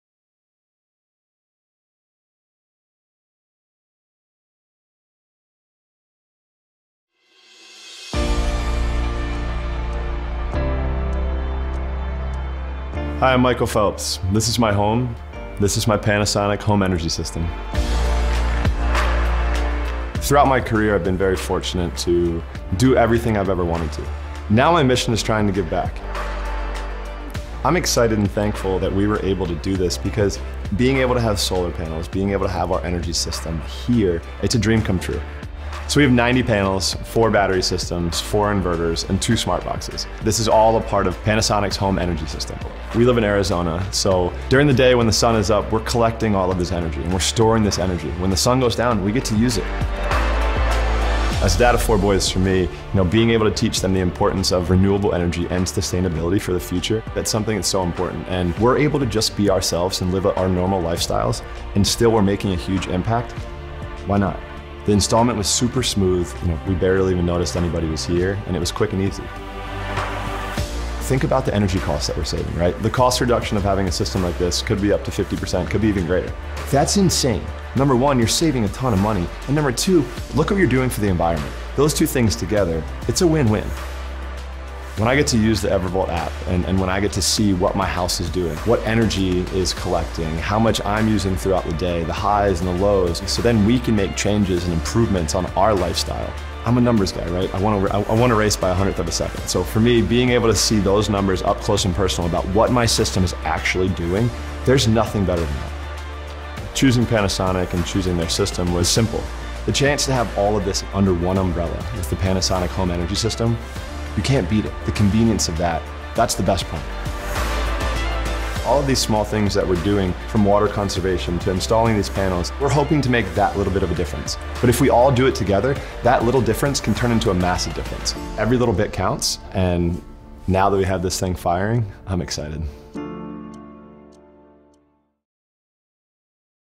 Olympic Champion & Team Panasonic Ambassador Michael Phelps Embraces Solar at Home with EVERVOLT®
The 23-time Olympic gold medalist has teamed up with Panasonic to convert his Arizona home to solar power as part of his commitment to building a more sustainable future for tomorrow.